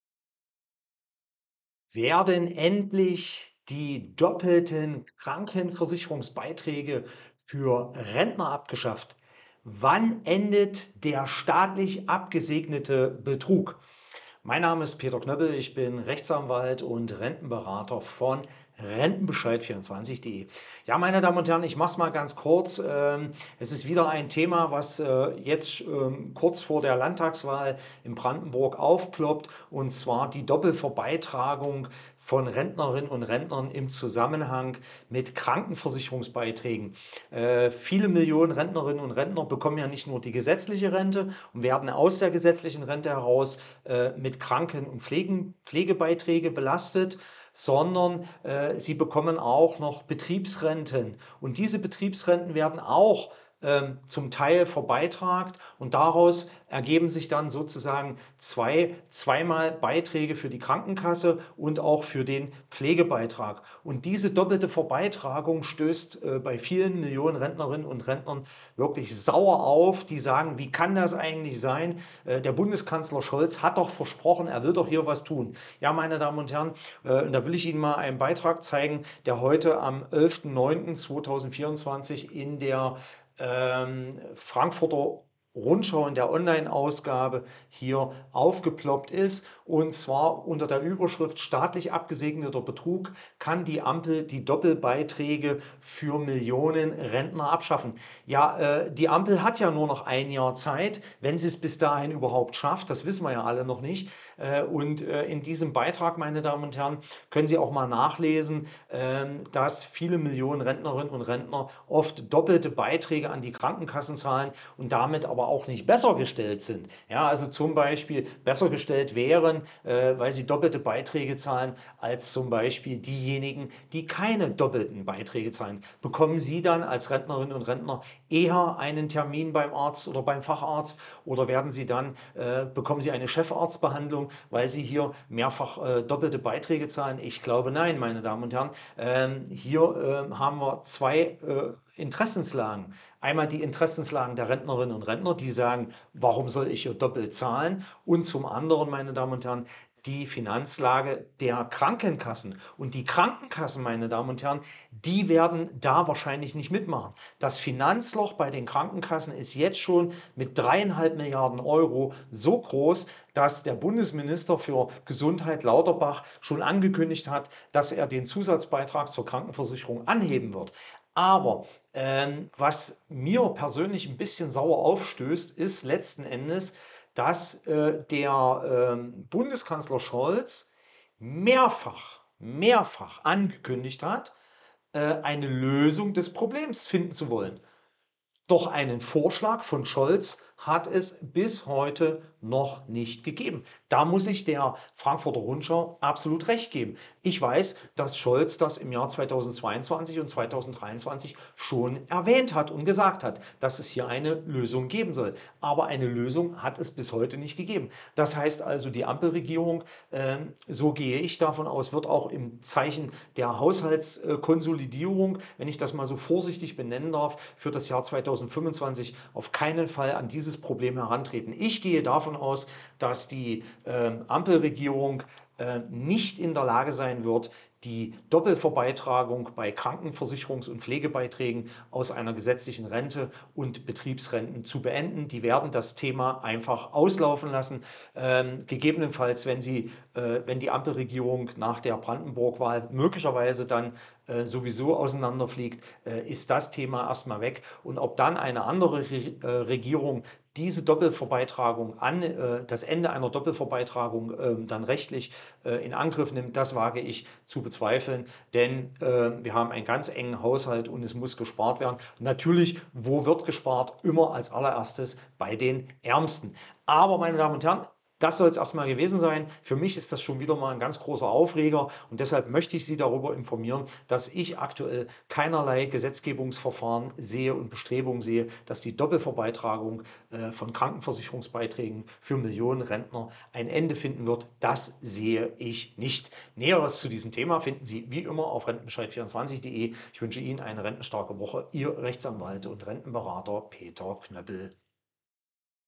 Kommt das Ende der Krankenkassen-Doppelbeiträge für Millionen Rentner
Die Frankfurter Rundschau titelte am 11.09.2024 in ihrer Online Ausgabe: „Staatlich abgesegneter Betrug“: Kann die Ampel die Doppelbeiträge für Millionen Rentner abschaffen?".
Es geht um ein Problem, was Millionen Rentnerinnen und Rentner nur noch wütend macht. Sie zahlen auf Ihre Rente Kranken-und Pflegebeiträge und bei Betriebsrenten noch einmal dazu. Doppelte Krankenkassenbeiträge für zwei Renten. Auf Grund von erheblichen Protest und Druck aus der Bevölkerung führte der Gesetzgeber einen Freibetrag bei den KV-und Pflegebeiträgen in der Betriebsrente ein. Dieser Freibetrag führt dazu, dass die Beitragsbelastung aus Krankenkassen-und Pflegebeiträgen geringer geworden ist, aber die doppelte Verbeitragung nicht aufhört. Kanzler Scholz ( SPD) hatte versprochen, eine Lösung des Problems auf den Tisch zu legen. Bis zum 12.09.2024 ist nach dem Kenntnisstand des Rentenberater Peter Knöppel aber nichts passiert. Ob die aktuelle Ampelregierung überhaupt noch eine Lösung bis zur Bundestagswahl im jahr 2025 anbietet, ist offen, weil niemand aktuell vorhersagen kann, ob es die Regierung nach der Landtagswahl in Brandenburg noch geben wird oder wenn ja, ob der Bundeshaushalt 2025 in die Kassenlage der Krankenkassen mit einem Milliarden-Defizit eine Änderung der Rechtslage zulassen. Wir sind gespannt, wie es weitergeht, mit dem Versprechen von Scholz eine Lösung vorzulegen. Es bleibt also weiterhin so, dass Millionen Rentnerinnen und Rentner weiter doppelt abkassiert werden.